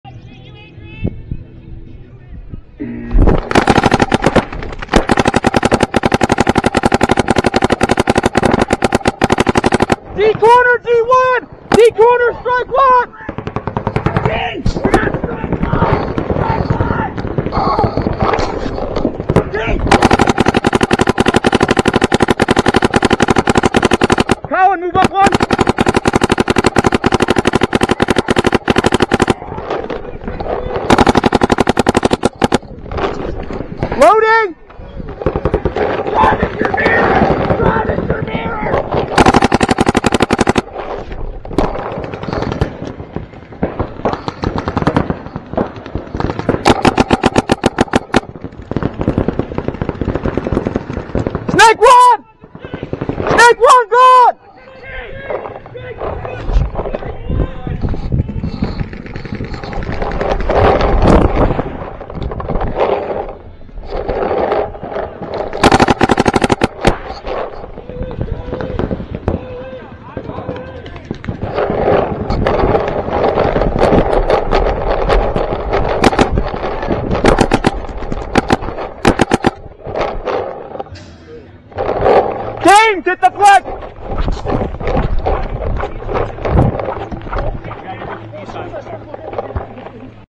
OTP - EPL Practice 2 - Quick win
Practice at  'ON TARGET PAINTBALL'  for EPL series 2020 Independence Cup
Scarp123 - camera man (D temple)
Smurf - 50 (right side)
Derek - 50 (left side)
Morgan - Snake
Colin _ D corner